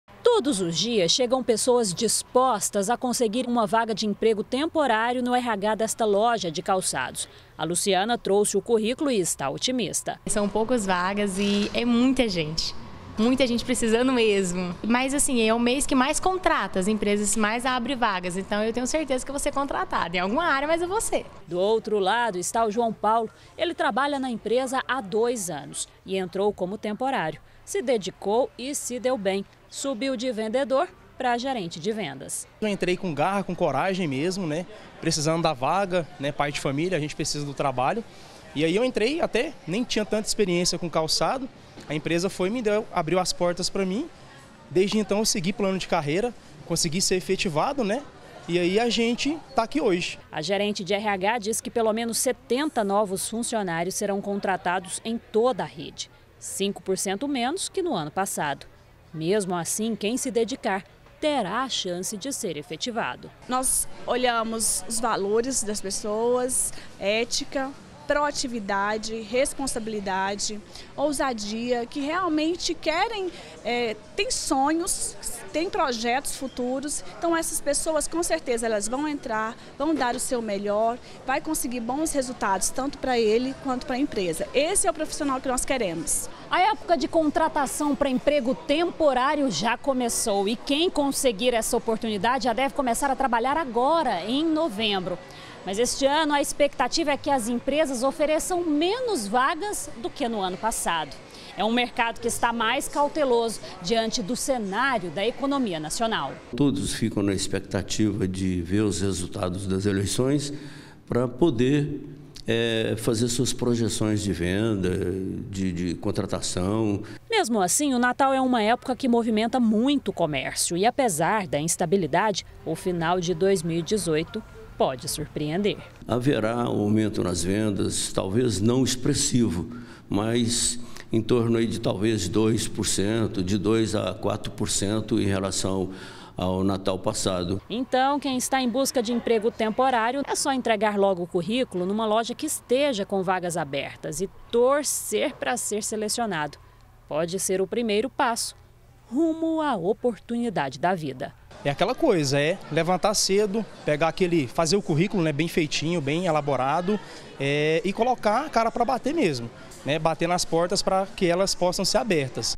GR - Emprego temporário: comércio está com vagas abertas - 25-10-2018
Pra quem tá a procura de emprego, uma boa oportunidade as lojas já está com vagas abertas pro fim de ano. A princípio são apenas vagas pra trabalho temporário, até o natal. Mas o funcionário que se dedica, tem chance de ser efetivado.